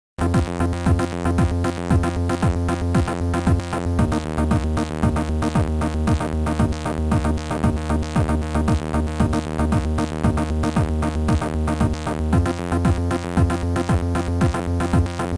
wow mike mateis mom is in a video game!!!who knew?
wow mike mateis mom is in a video game!!!who knew? Yep she sure is to.I wonder what avgn thinks of this.I wonder if she knows about cheetah men 2.haha who knows?This is just a joke guys I hope I get a laugh. I'm a big avgn fan to.please enjoy.